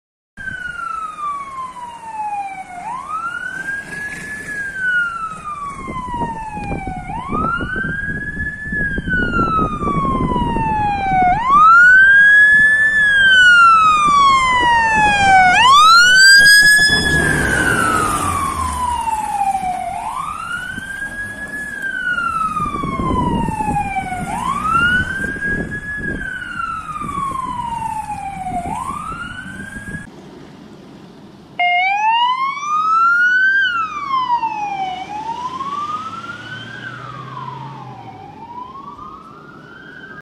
[Alarmowo] 319[P]53 Scania P410 GCBARt z OSP Golęczewo do pożaru krzaków w Chludowie
✔️ROZWIŃ OPIS✔️

🚨 ALARM OSP Chludowo 🚨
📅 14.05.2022
🕑 14:29
⚠️ Pożar Krzaków
📌 Chludowo Ul. Strumykowa
SiŚ na miejscu zdarzenia:
🚒 337[P]66 - GCBARt 5/32 Scania P410/WISS - OSP Chludowo
🚒 319[P]53 - GCBARt 8/50 Scania P410 XT/WISS - OSP Golęczewo

▶️SOCIALMEDIA◀️